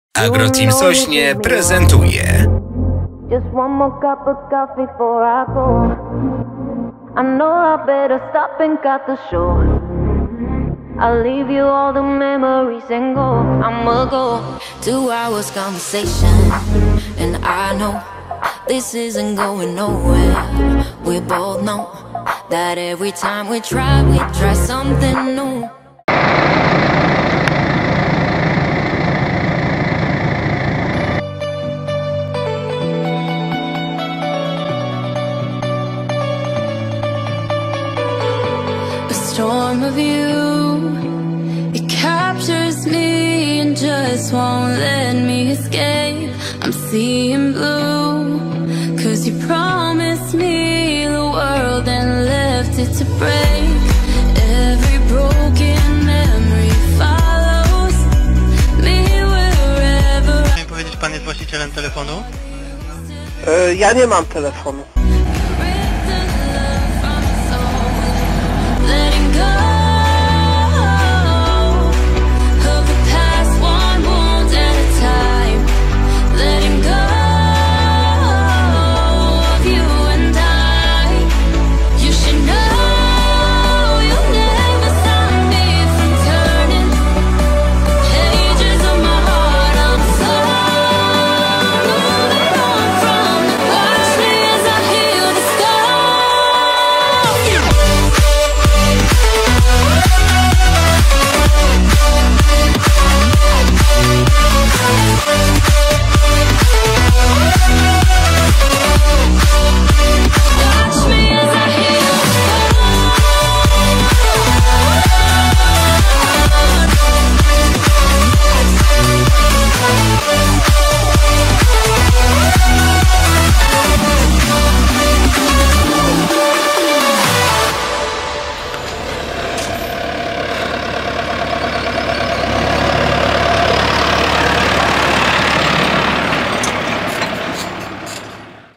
☘️Sianokiszonka na pustyni z najlepszą ekipą!☘️Wieczorny klimat☘️Green Power!☘️
Zachęcamy do zostawienia łapki w górę komentarza oraz subskrypcji bo to nas motywuje do dalszej pracy.
Dziś przedstawiam wam moją najnowszą produkcję w której są przedstawione sianokiszonki w 2 gospodarstwach!

Ścieżka dźwiękowa nie jest moją własnością i nie roszczę sobie do niego praw autorskich. Film ma charakter edukacyjny. 😀